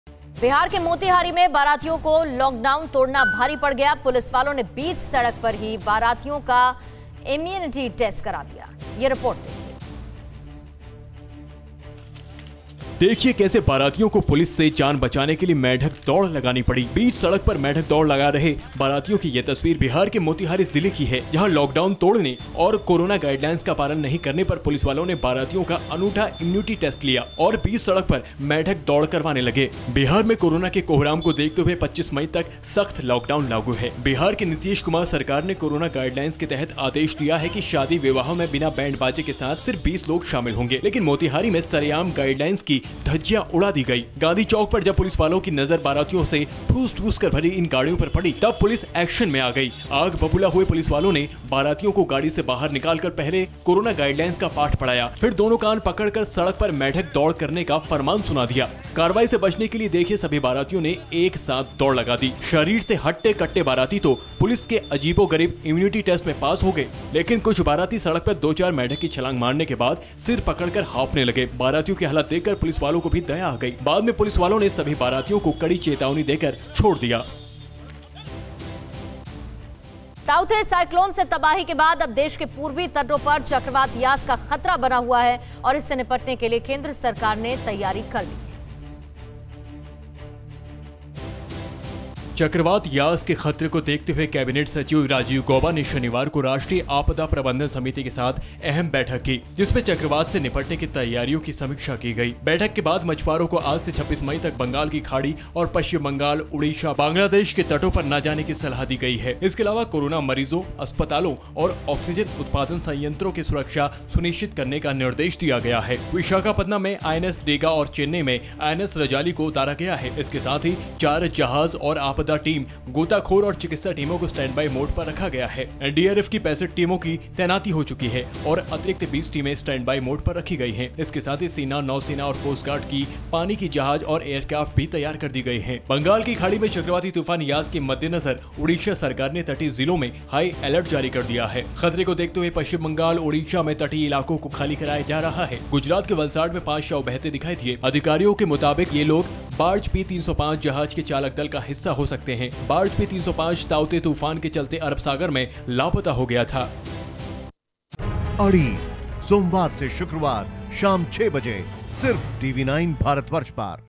Bihar : बीच सड़क पर बारातियों का Immunity Test! | Lockdown Violation
बिहार के मोतिहारी में बारातियों को लॉकडाउन तोड़ना भारी पड़ गया. पुलिसवालों ने बीच सड़क पर ही बारातियों को अनोखी सजा दी. देखिए ये रिपोर्ट...

Credit